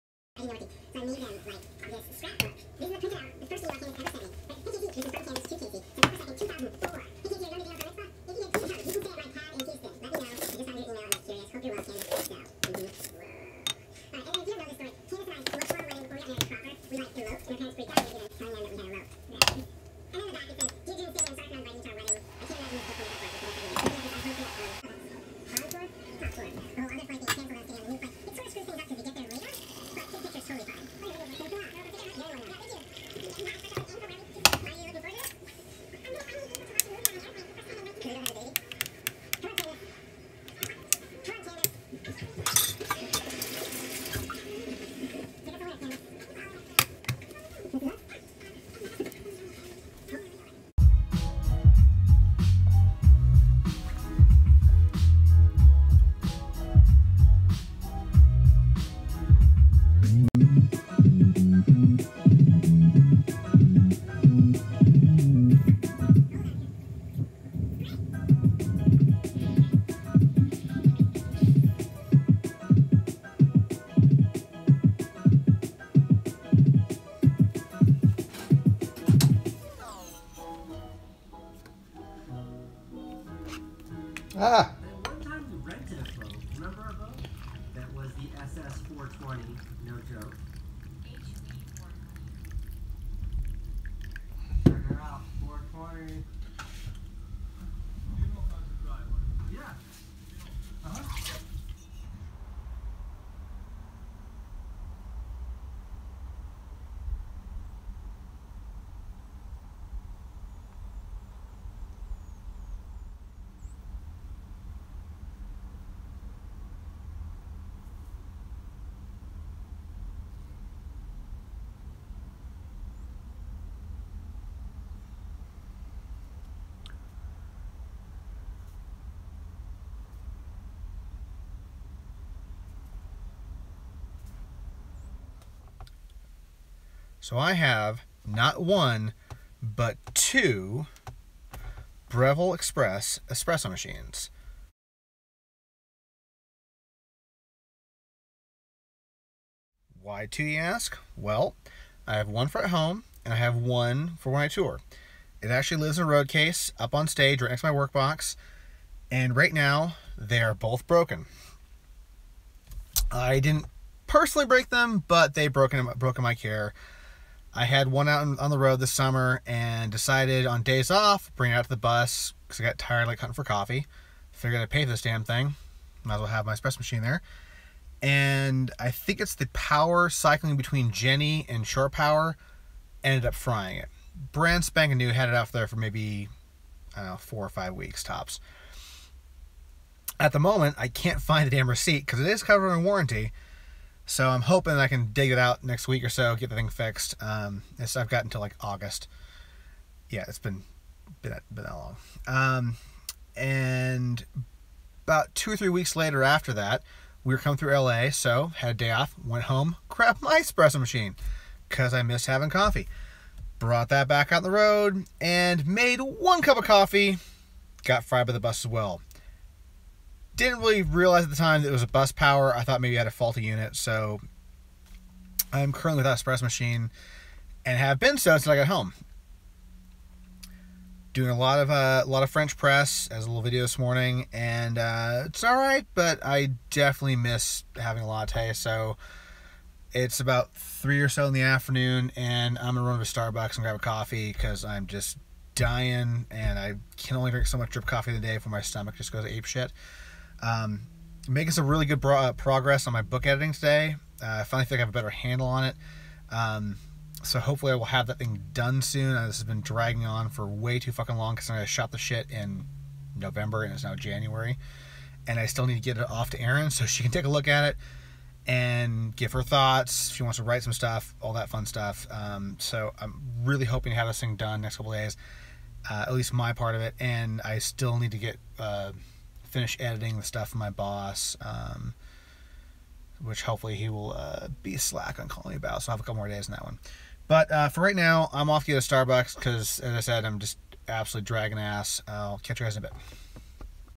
January 8, 2017 Episode 027 I Miss My Breville
Two broken Breville Barista Express espresso machines and I'm stuck with a French press.